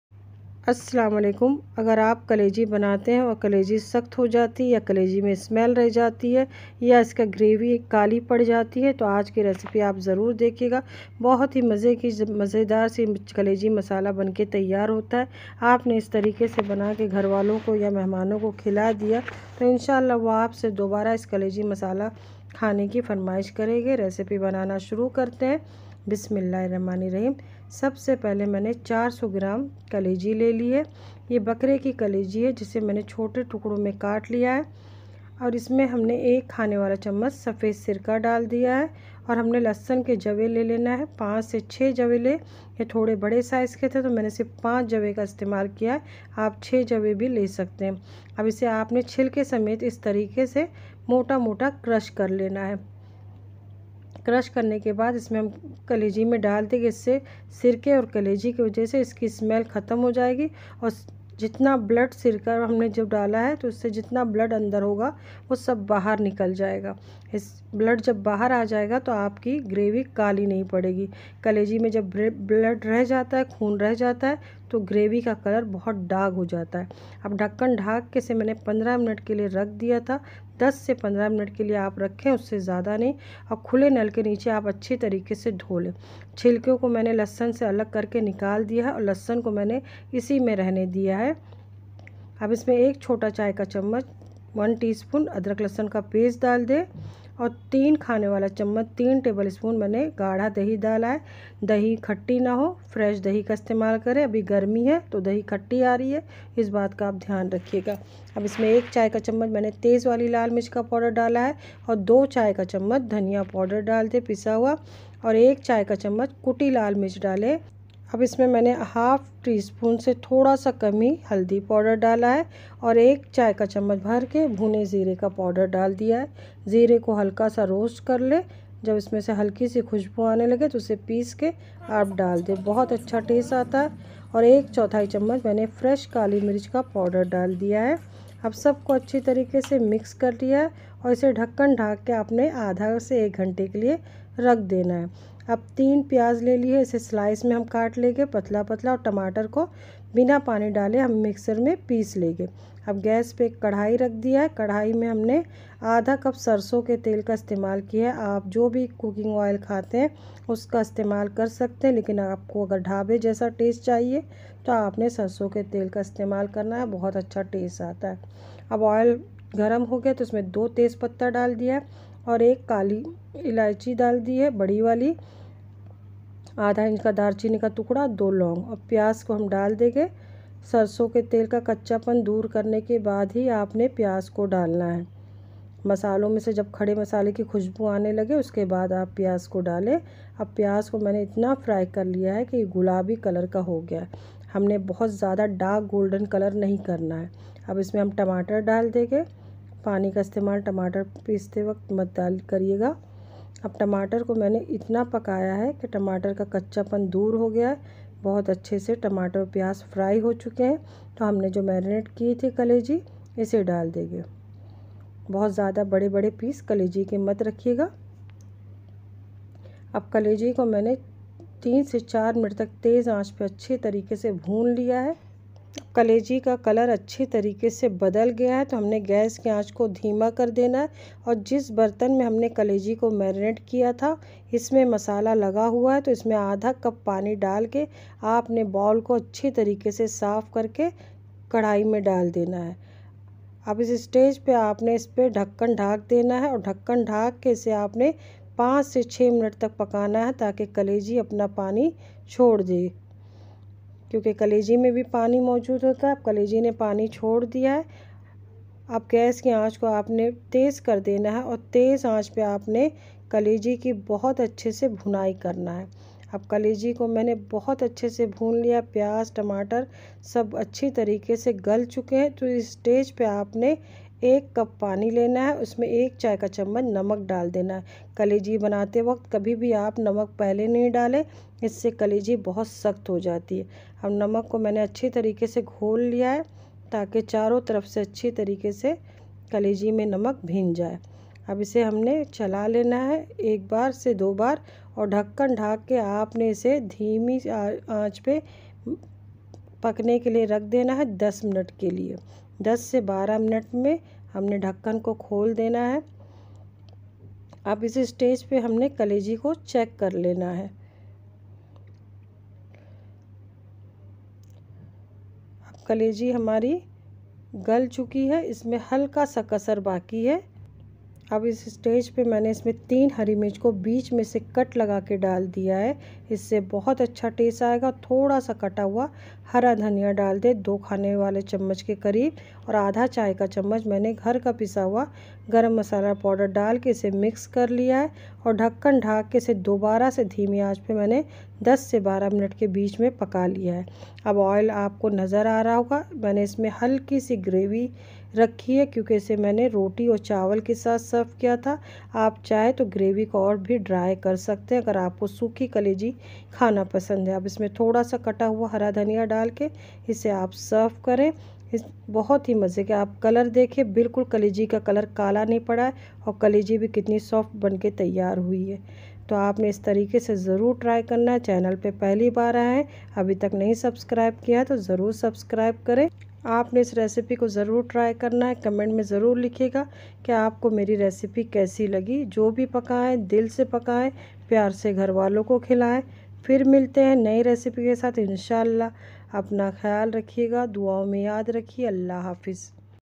Dhaba Style Chatpati Kaleji Masala | Aisi Soft Kaleji Banae Khane Wale Ko Maza Aa Jae
Ingredient
400gm kaleji and Gurda (Liver ,Kidney)
1tbs vinegar
6 Garlic cloves Crushed
1tsp Ginger garlic paste
3tbs Curd
1tsp Red chilli powder
1tsp Red chilli Flakes
1/2 tsp Turmeric powder
2 tsp Coriander powder
1/4 tsp Black pepper powder
1 tsp Roasted Cumin powder
1/2/cup Mustard oil
 2 bay leaf
1 Black cardamom
2 Cloves
1/2 inch cinnamon stick
3 Medium size Onion slice
1 Tomato
1/2 tsp Garam Masala powder
salt to taste
3 Green Chilli
few chopped coriander Leaves